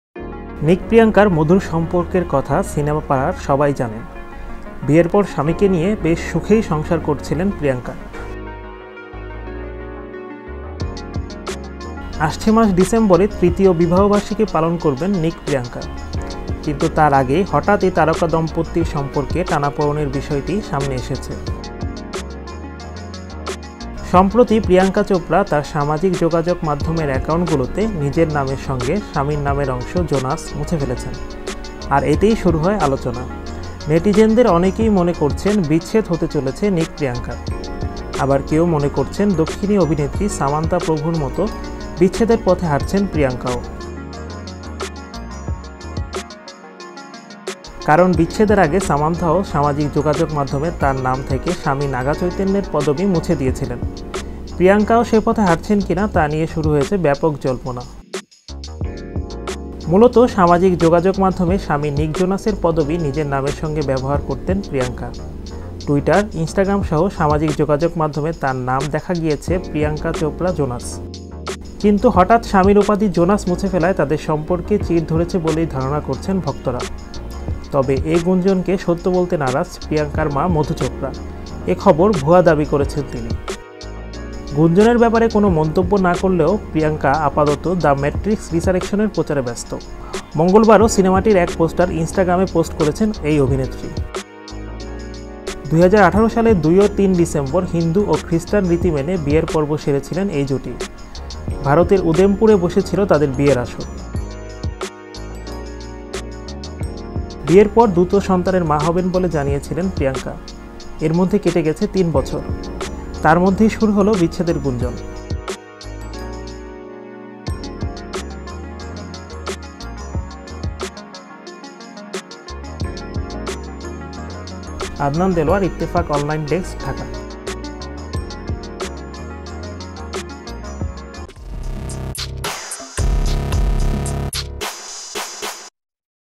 বিচ্ছেদের গুঞ্জন নিক-প্রিয়াঙ্কার
সম্প্রতি প্রিয়াঙ্কা চোপড়া তার সামাজিক যোগাযোগমাধ্যমের অ্যাকাউন্টগুলোতে নিজের নামের সঙ্গে স্বামীর নামের অংশ ‘জোনাস’ মুছে ফেলেছেন। আর এতেই শুরু হয় আলোচনা।